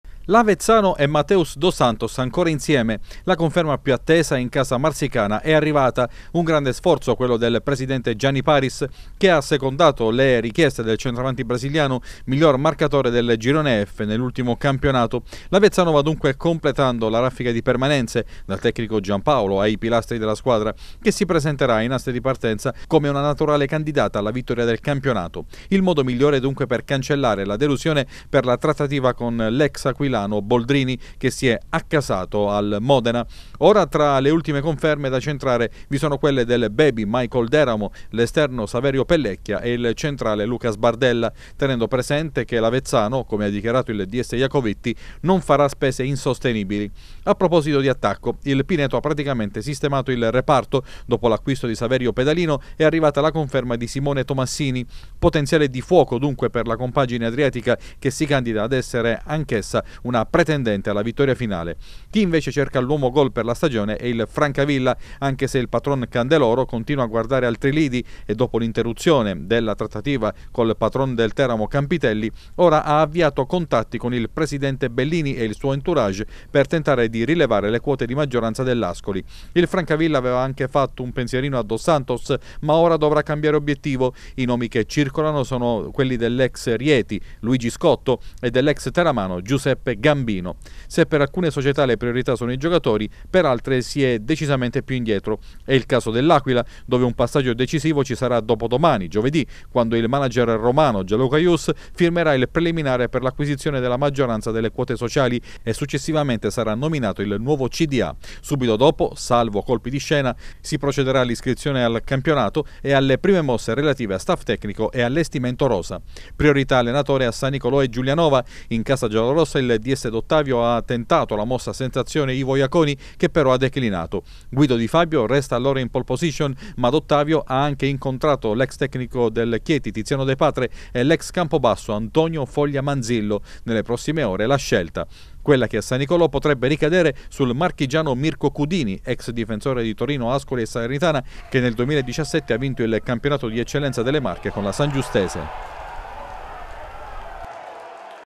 Dos Santos resta ad Avezzano Il Pineto si tiene Tomassini
Anche il prossimo anno Mateus Dos Santos guiderà l’attacco dell’Avezzano. Il presidente Paris proprio in queste ore ha strappato il sì al centravanti brasiliano. Una importante conferma anche a Pineto, quella del centravanti Simone Tomassini